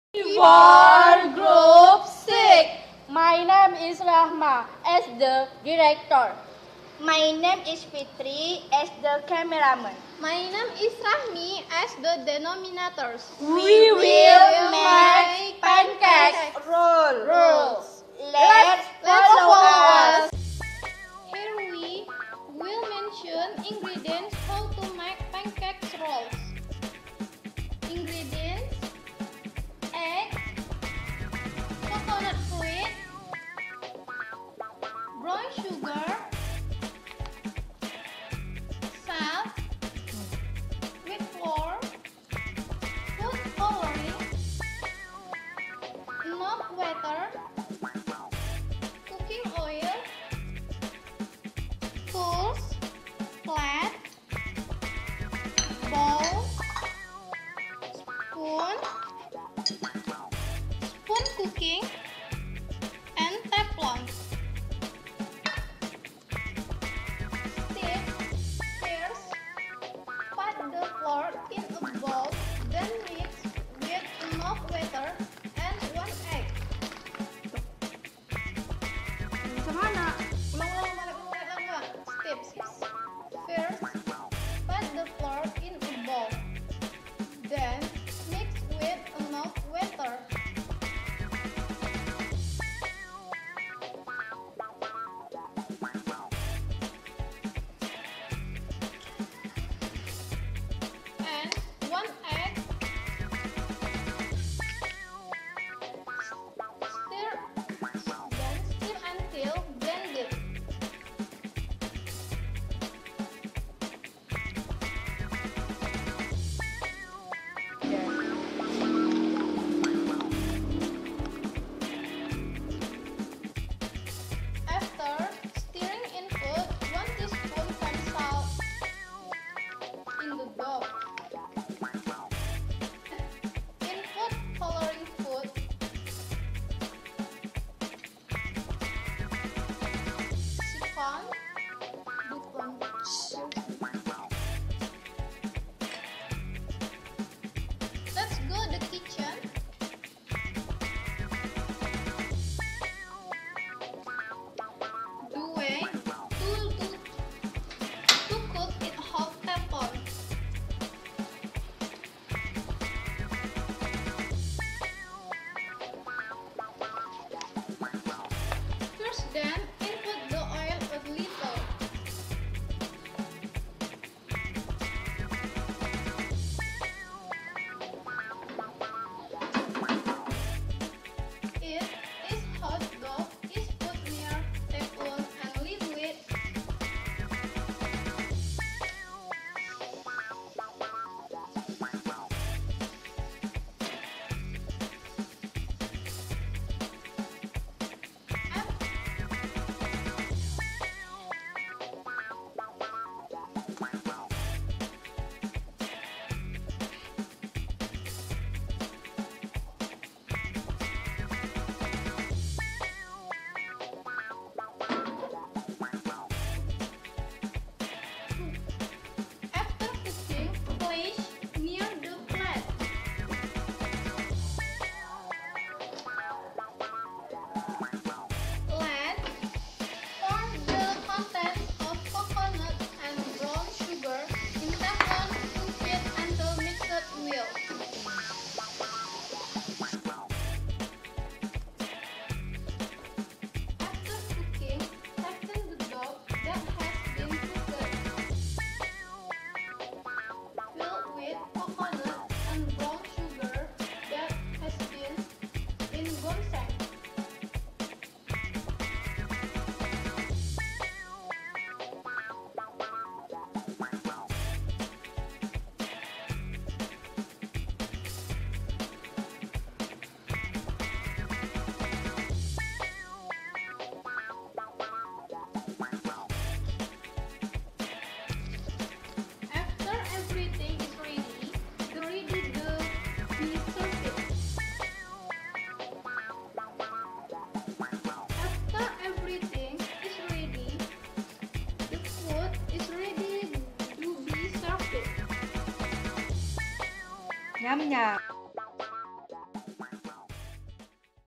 KELOMPOK 6: SMP BUDI DHARMA DUMAI
TUGAS PROCEDURE TEXT DARI SMP BUDI DHARMA DUMAI TAHUN 2017